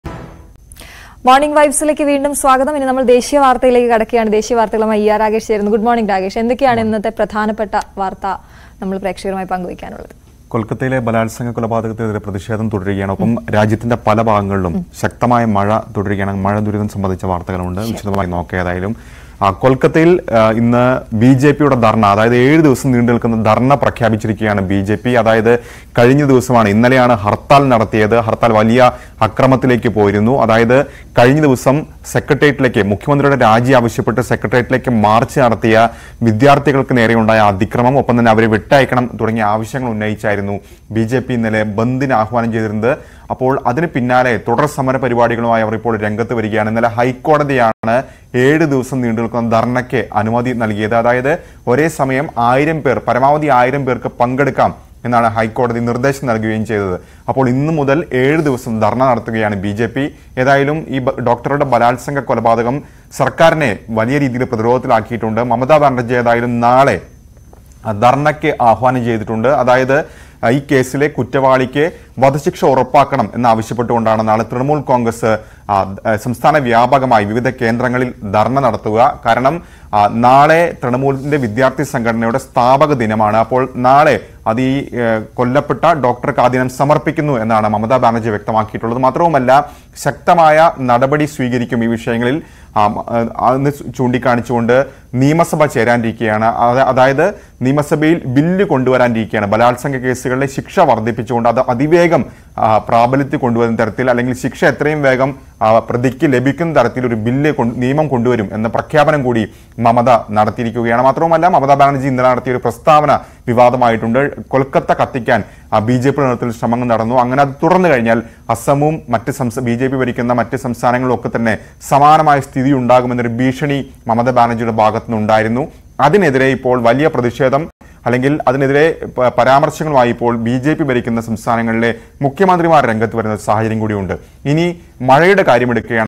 Kolkata Doctor Murder | ജൂനിയർ ഡോക്ടറുടെ കൊലപാതകം ; കൊൽക്കത്തയില്‍ പ്രതിഷേധം തുടരുന്നു | BJP Protest
Kolkata Doctor Murder : കൊൽക്കത്തയിലെ കൊലപാതകത്തിലും, പ്രതിഷേധിച്ചവർക്ക് നേരെ ഉണ്ടായ പൊലീസ് നടപടിയിലും പ്രതിഷേധം തുടരുന്നു. ബിജെപിയുടെ ഏഴുദിവസം നീണ്ടു നിൽക്കുന്ന ധർണയ്ക്ക്  ഇന്ന് തുടക്കമാകും. കൊലപാതകത്തിലെ പരാതിയ്ക്ക് വധശിക്ഷ നല്കണമെന്ന് ആവശ്യപ്പെട്ട് മുഖ്യമന്ത്രി മമത ബാനർജി നാളെ ധർണ ആഹ്വാനം ചെയ്‌തിട്ടുണ്ട്.

Protests continue in Kolkata over the murder and the police actions against the demonstrators. The BJP will begin a seven-day protest today. Chief Minister Mamata Banerjee has called for a demonstration tomorrow, demanding the death penalty for those involved.